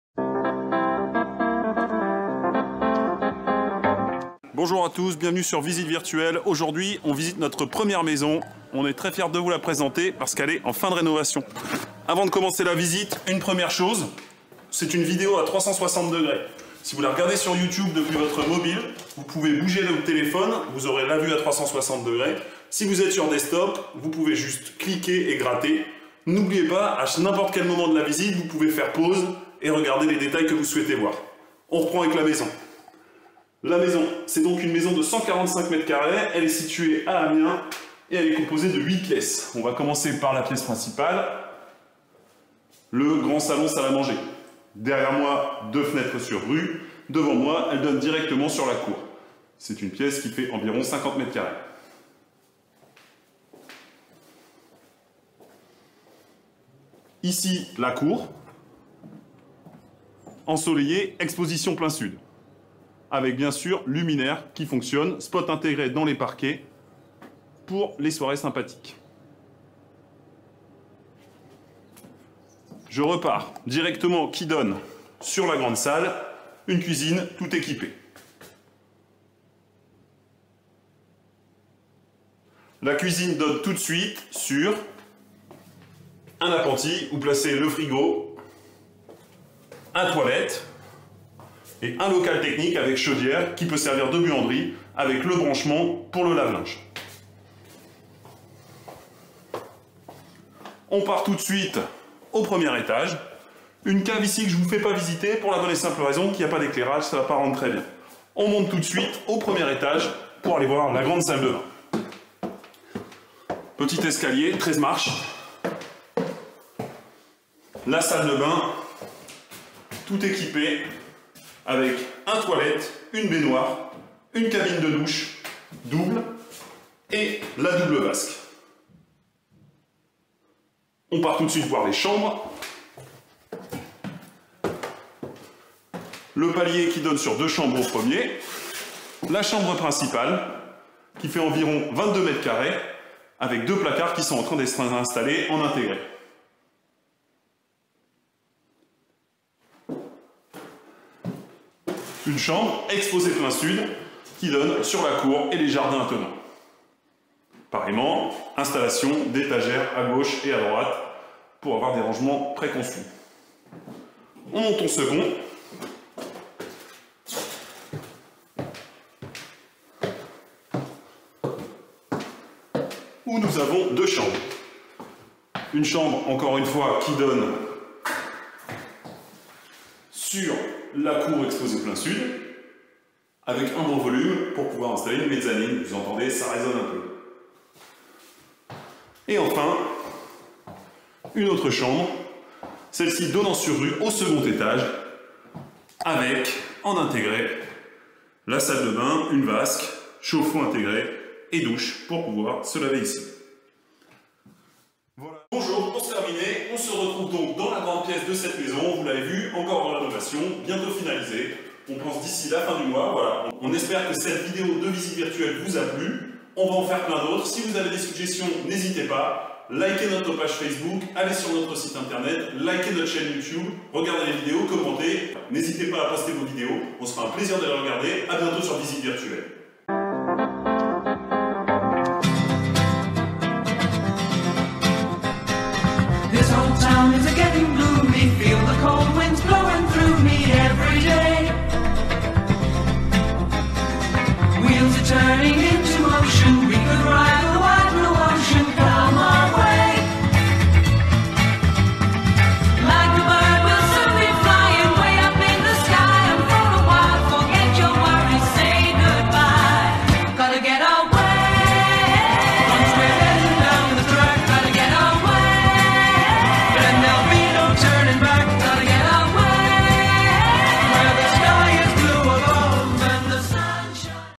Visite virtuelle: Maison Amiens, 8 pièces, 4 chambres [ VR 360 ]
Aujourd'hui, nous vous présentons la visite virtuelle d'une charmante maison Amiénoise 8 pièces mise en location.

Pour profiter de la vidéo au maximum:
1- Débutant: utilisez l'appli ou le site Youtube.Déplacez l'image en cliquant avec votre souris.
2- Fan: casque en carton à mettre sur votre téléphone
3- Rêveur: casque plastique à mettre sur votre téléphone
4- Adorateur: casque réalité virtuelle